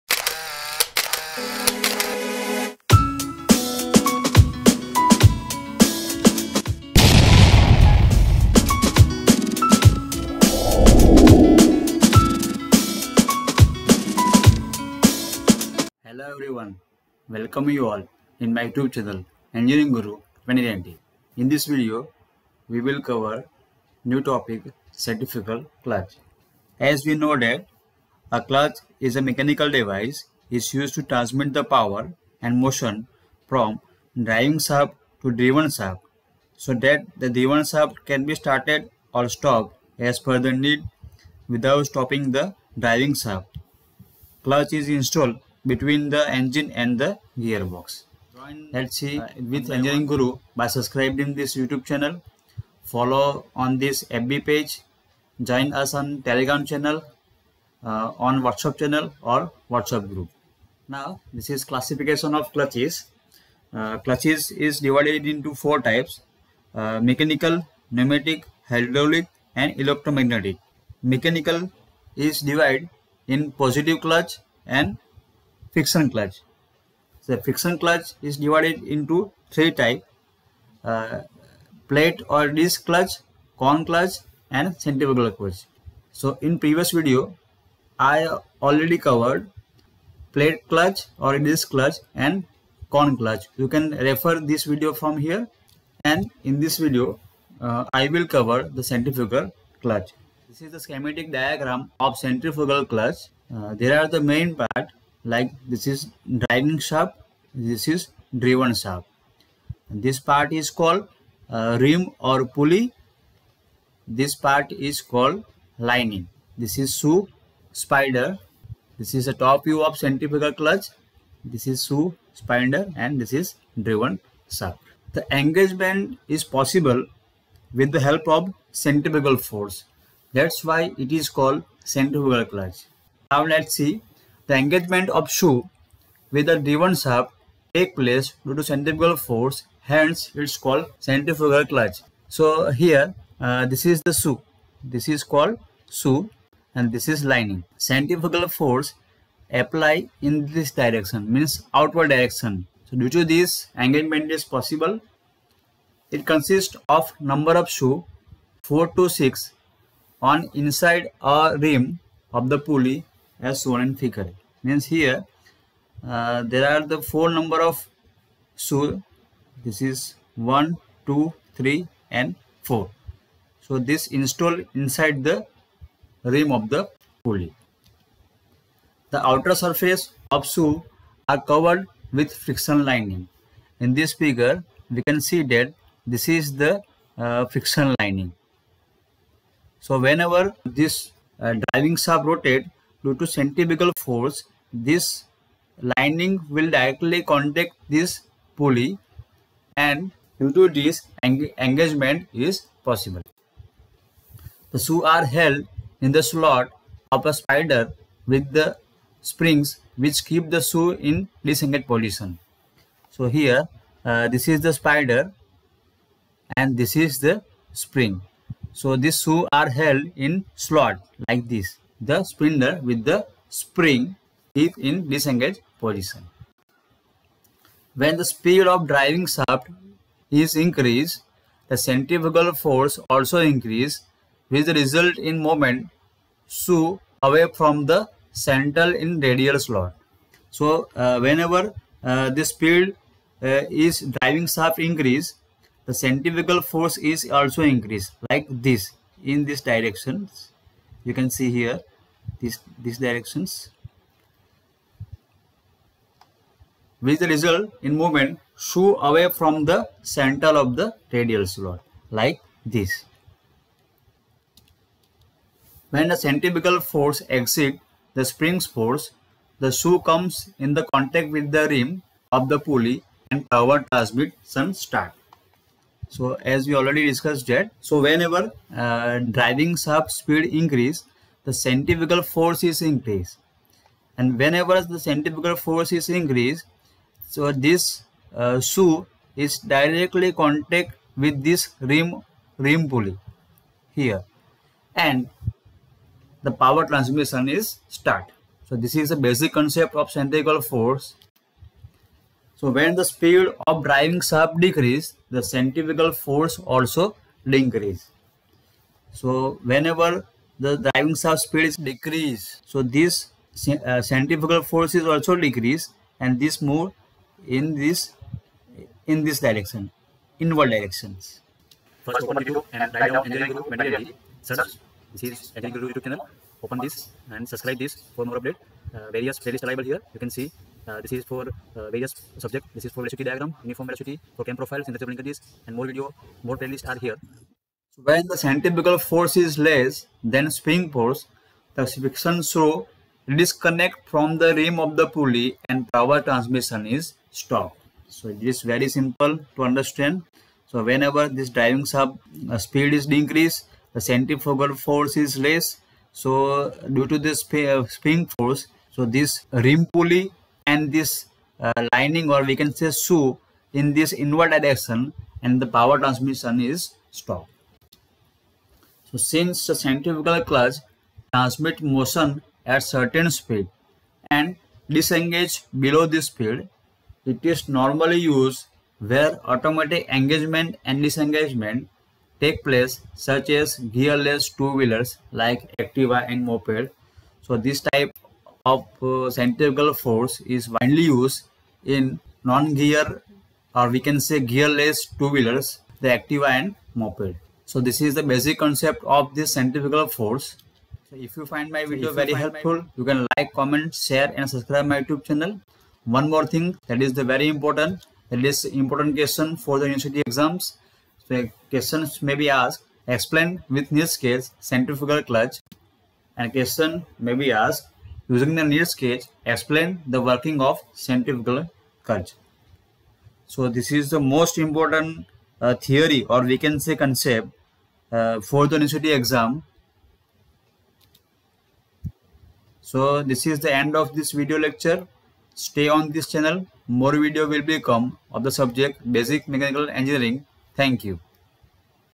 3| Centrifugal Clutch
In this video, You will learn Single Plate Clutch with the following topic.

1. What is Centrifugal Clutch?

2. purpose of Centrifugal Clutch.

3| Centrifugal Clutch